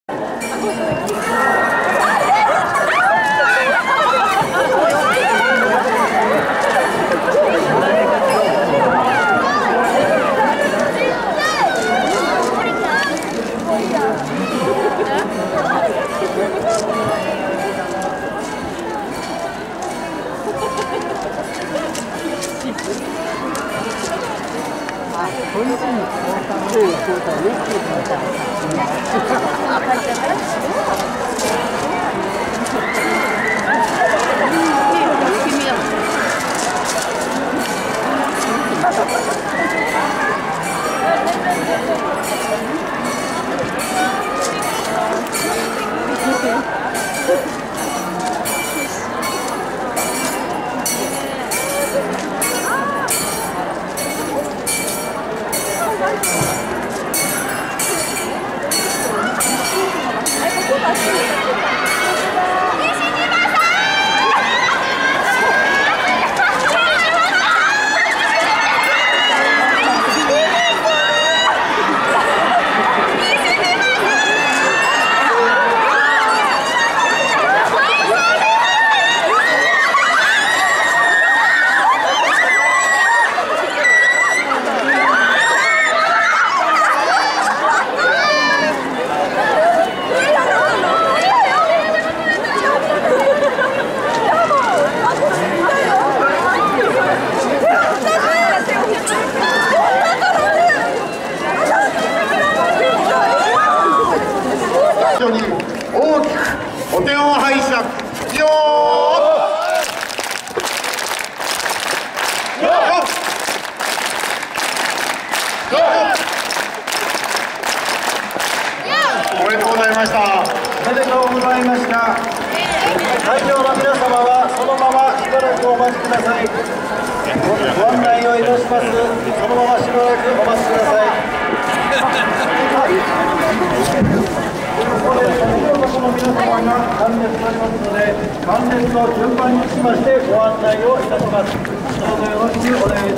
『奥様は、取り扱い注意』 綾瀬はるか 西島秀俊 が成田山で豆まき。私も次の回でまいたんです『福は内』節分
今年の節分は大本堂前に特設舞台を設置するのをやめたようなので、2013年の動画をアップしました。

大河ドラマ「八重の桜」のキャスト 綾瀬はるか 西島秀俊 長谷川博己 剛力彩芽 小泉孝太郎 が豆まきしたあと降りてきました。